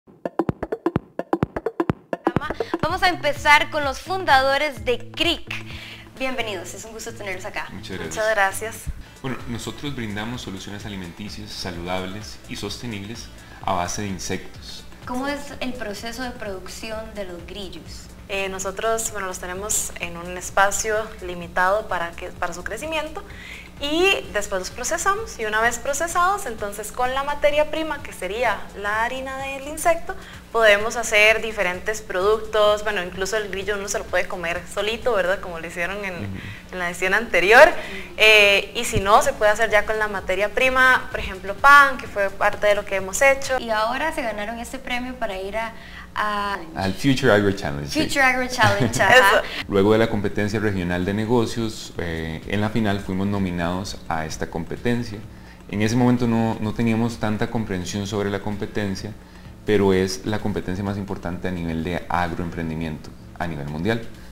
"CRIC" empresa de productos hechos con grillos representará Costa Rica en el "Future Agro Challenge"
Este domingo 23 a las 09:00 a.m. en Curiosidades vea todos los detalles de los productos que ofrece "Costa Rica Insect Company" y como se preparan para ir a Grecia al "Future Agro Challenge" la competencia más importante del sector agro en el mundo!

24 Horas conectados con la información